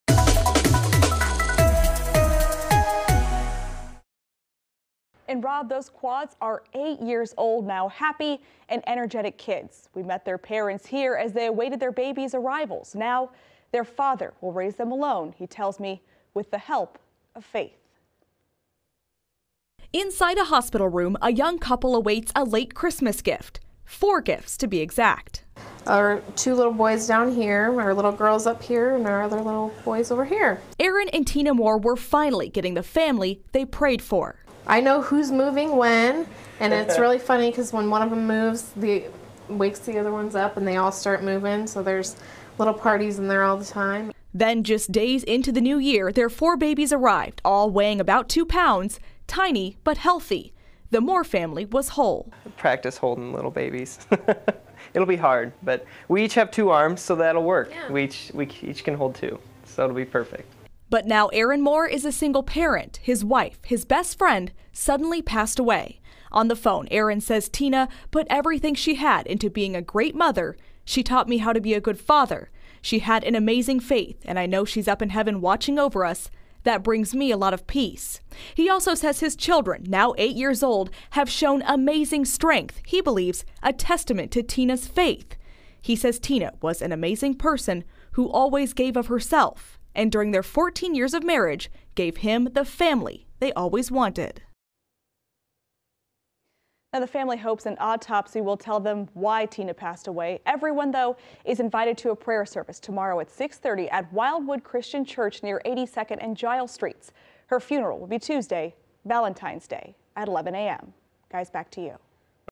Mother of four unexpectedly dies
Kristyna Engdahl reports on the mother of quadruplets, who died at 36.